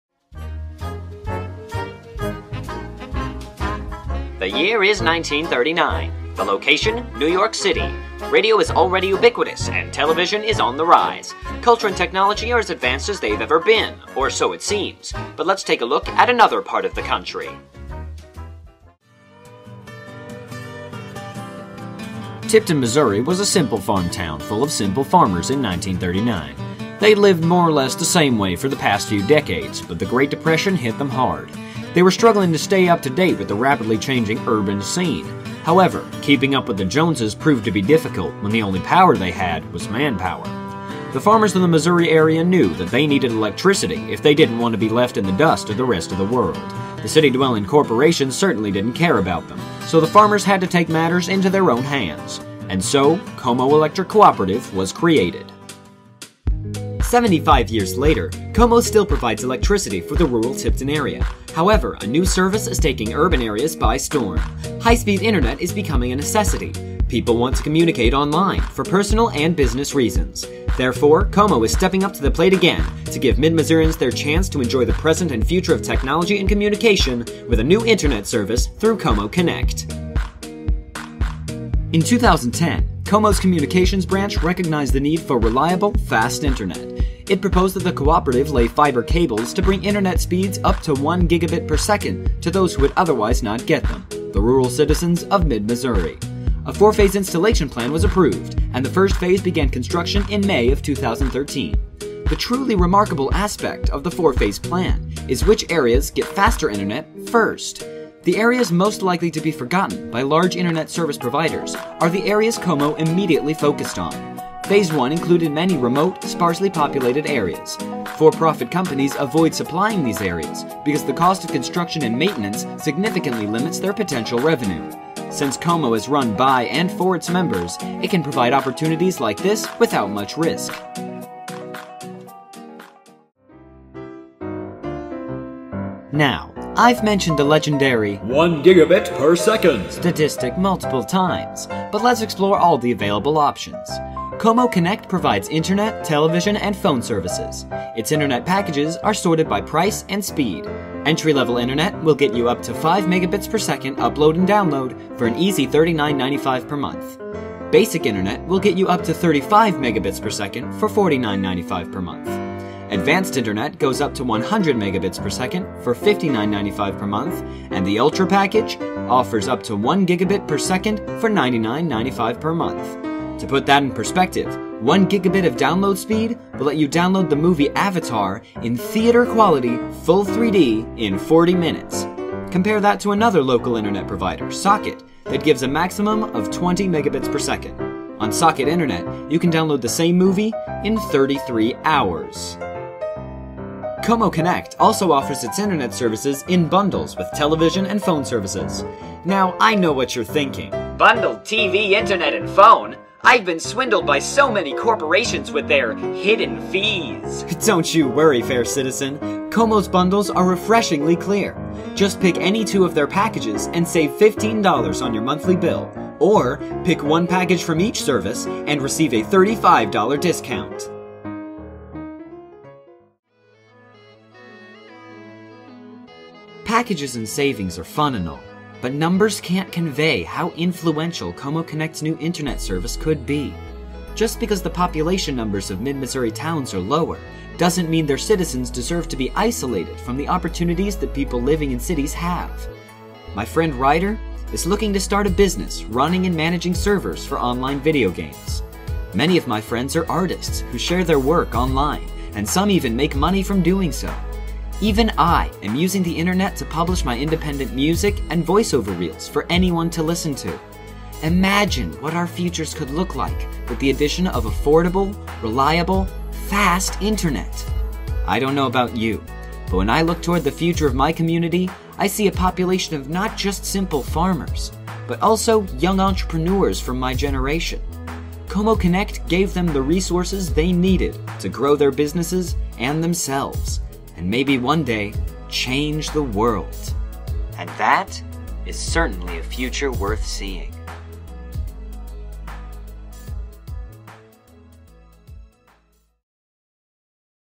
Youth Tour Finalist Project
Check out one of the projects judges scored highly enough to earn the creator a spot in the 2015 Youth Tour Finals, to be held Nov. 20, 2014.